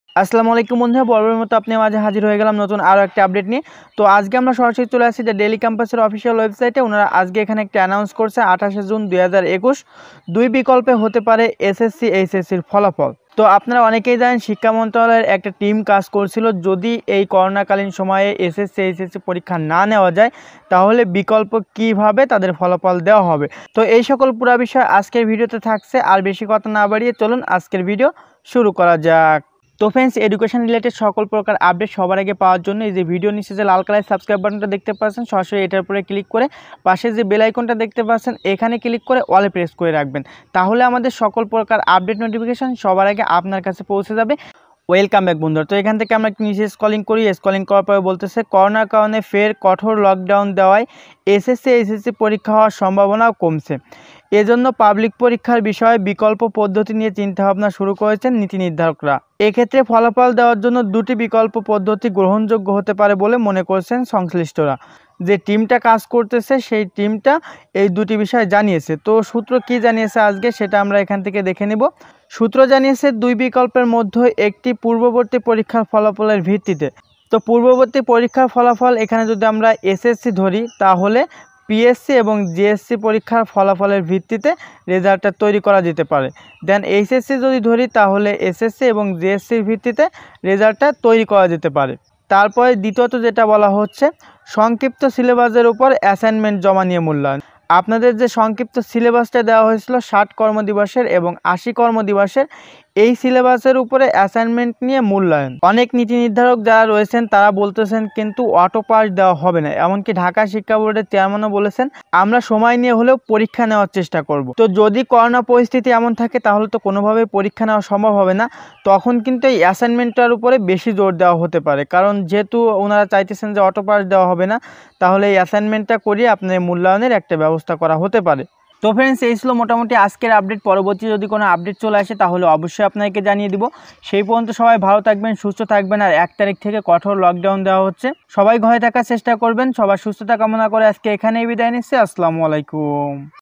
মন্ত্রণালয় জানালো দুই পদ্ধতিতে হতে পারে SSC_HSC পরীক্ষা || SSC 2021 || Learning Guide
✔সবার আগে সকল ধরনের আপডেট পেতে অনুগ্রহ করে আমাদের টেলিগ্রাম চ্যানেলে join করুন,,,

This is Learning Guide youtube channel, here is the new online video of new online income।